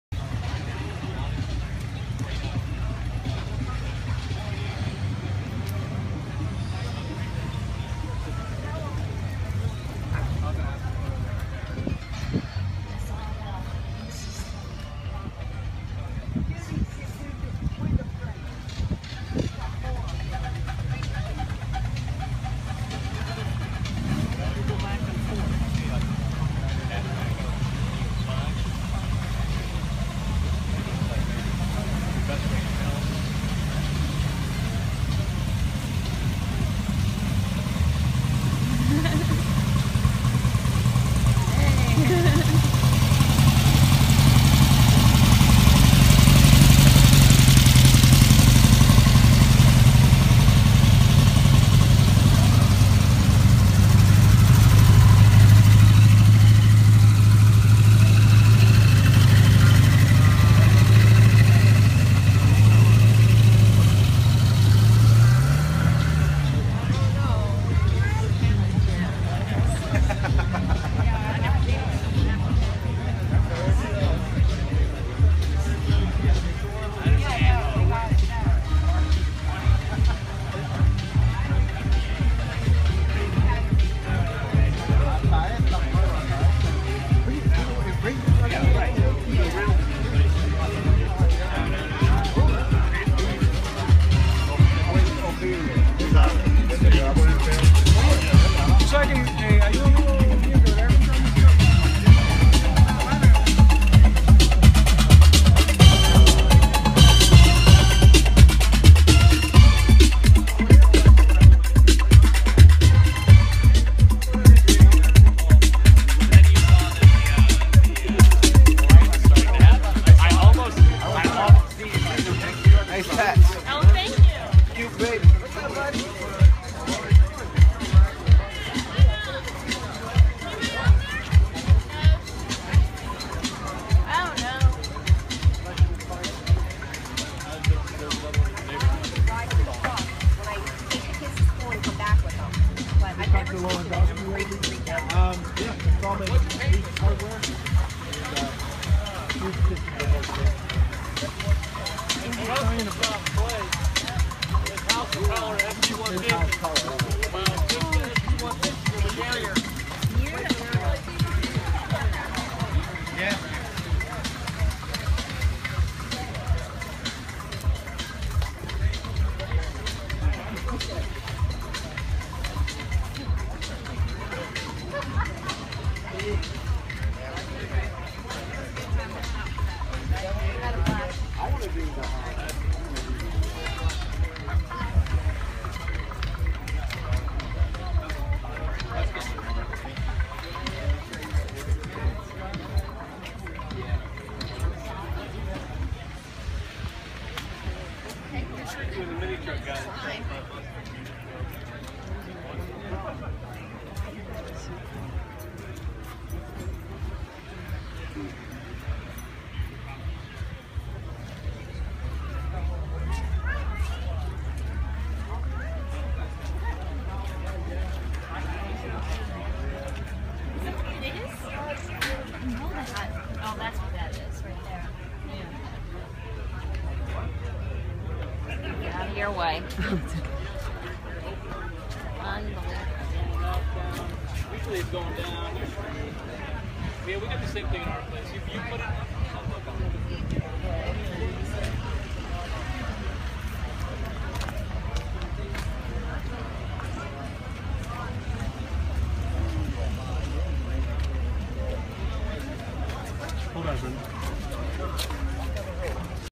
No Dough Bus Show April 2015 - Port Orange FL, VW Volkswagen Bus and VW Beetle Show. Groovy!
Volkswagen VW Bus, Beetle, Westfalia, Riviera, Camper annual meetup. No Dough Bus Show - April 2015 - Port Orange FL